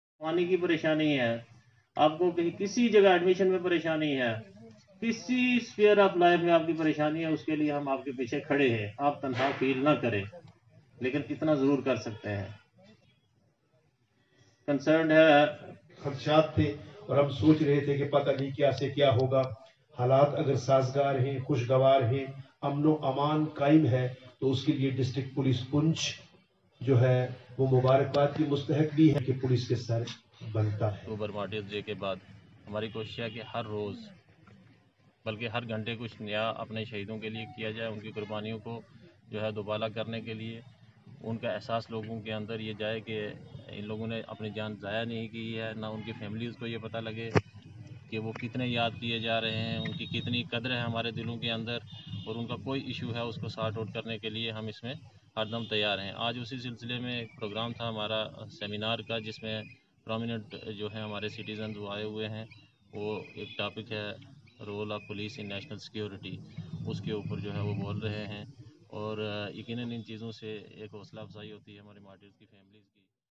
District Police Poonch is organizing a seminar on "National security"
*ABD NEWS 24X7*
*RAJIV SHARMA POONCH*

In the name of Martyrs and to preserve  unity, integrity and security of the nation,  District Police Poonch is organizing a seminar on "National security" , followed by honoring children's of police Martyrs